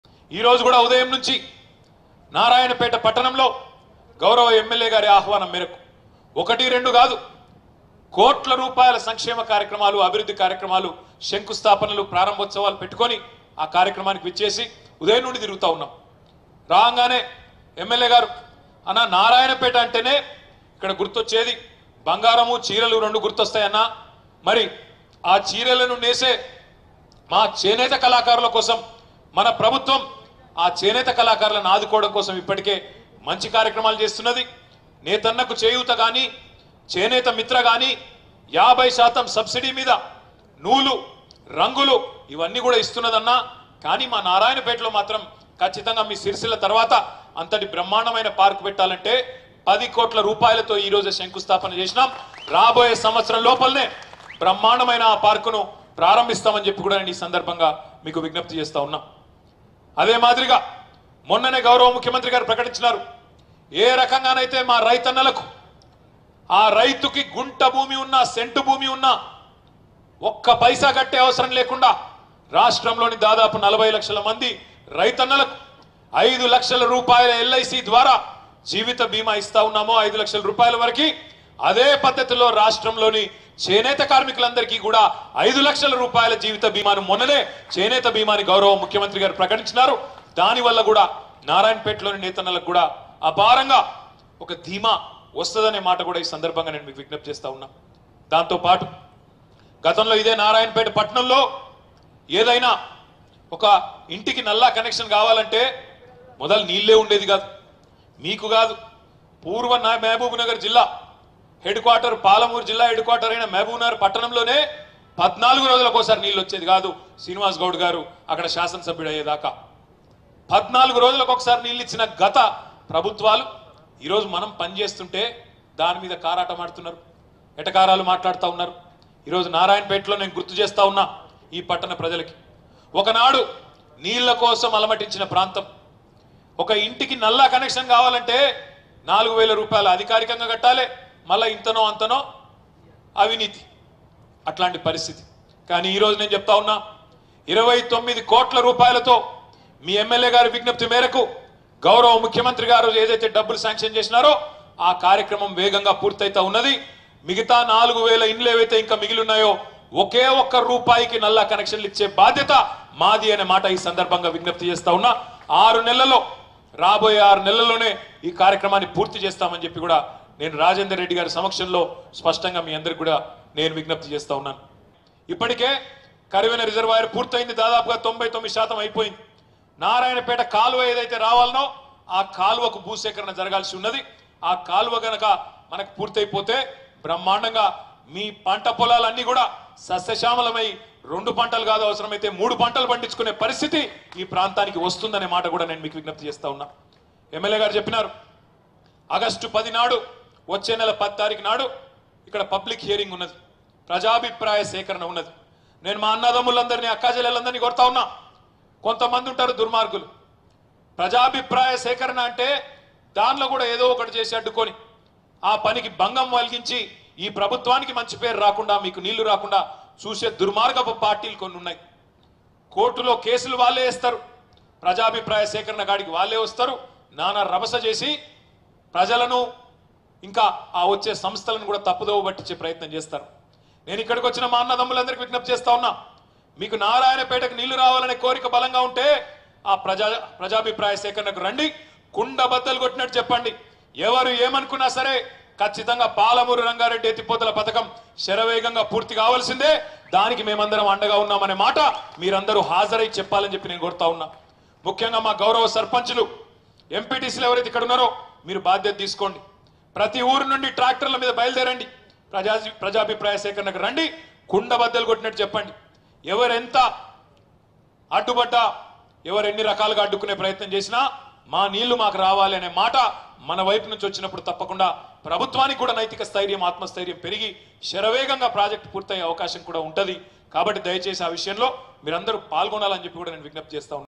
గతంలో 14 రోజులకు ఒకసారి .. నేడు రోజు తప్పించి రోజు మంచినీరు | Minister KTR Speech At Narayanpet
Minister KTR laid foundation Stone for Integrated Handloom Training & Textile Park in Narayanapet | T News